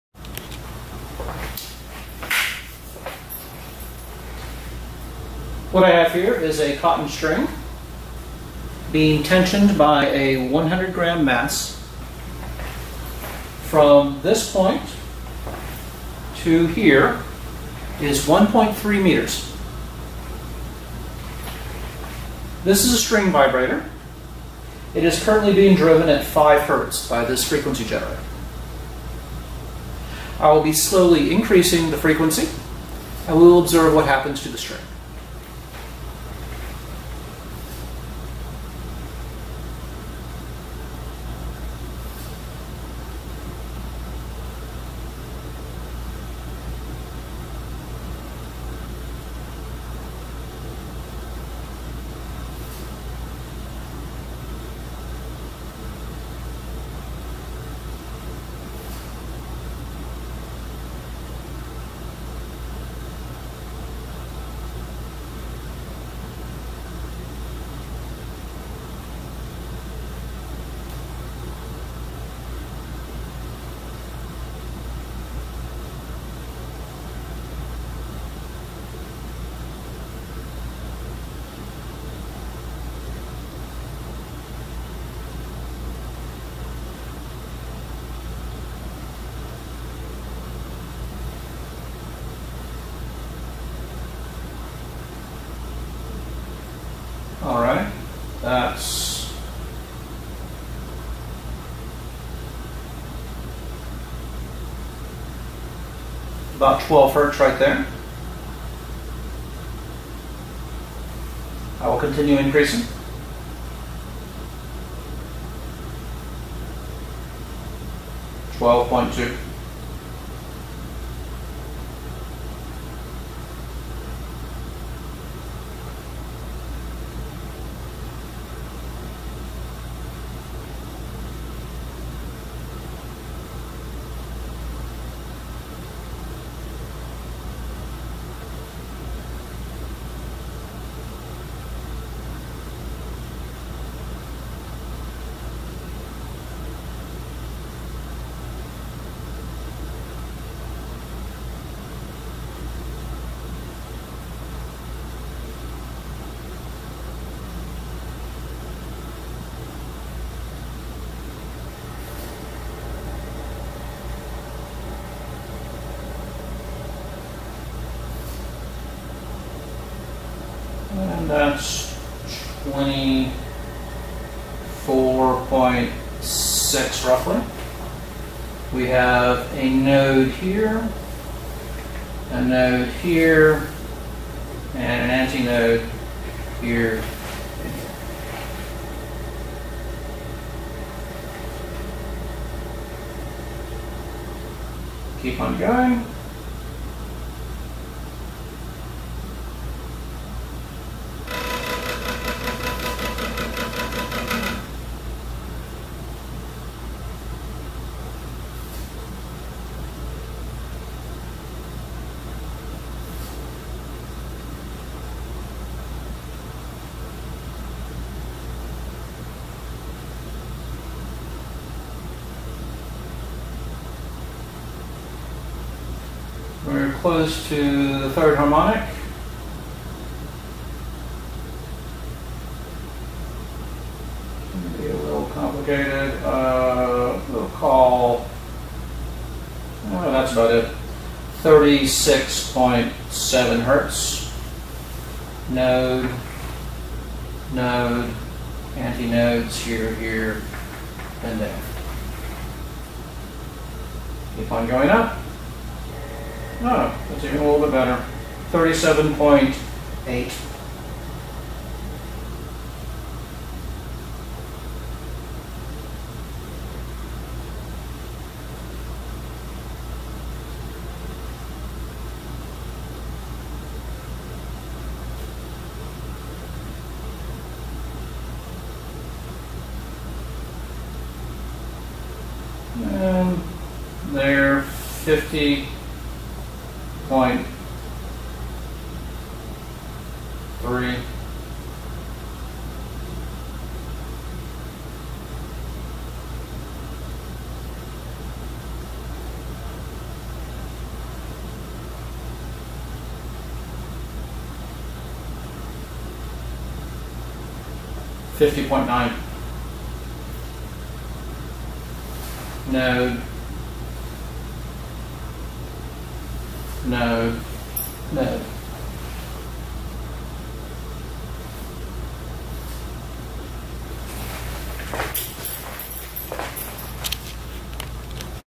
Driven Waves on a String
Demonstration of standing waves on a tensioned string using a vibrator and a frequency generator.
The string has a linear mass density of 0.00103 kg/m and is tensioned with a 100 g mass. The total length of the vibrating segment of the string is 1.3 m.